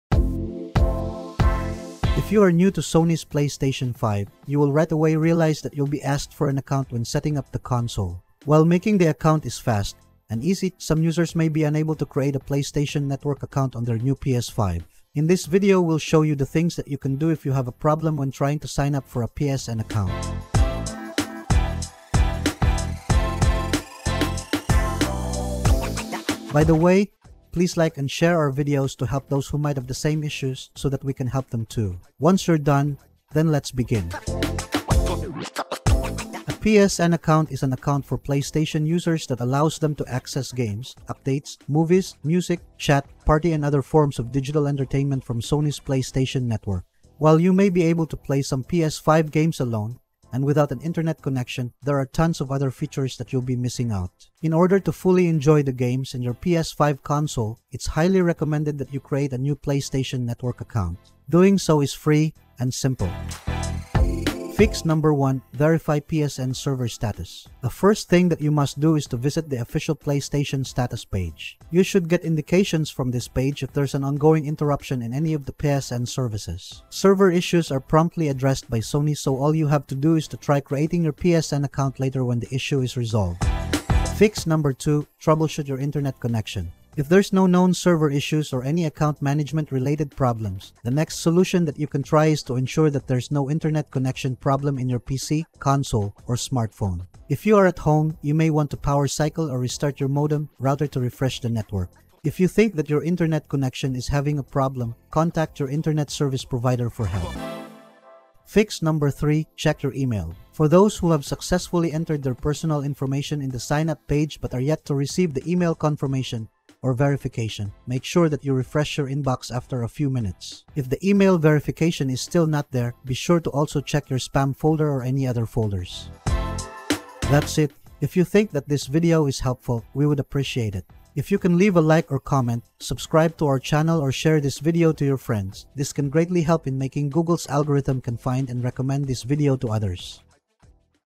How To Fix Unable To Create PSN Account On PS5 [Updated 2024]
If you are new to Sony’s PlayStation 5 (PS5), you will right away realize that you’ll be asked for an account when setting up the console. While making the account is fast and easy, some users may be unable to create a PlayStation Network (PSN) account on their new PS5.
In this video we’ll show you the things that you can do if you have a problem when trying to sign up for a PSN account.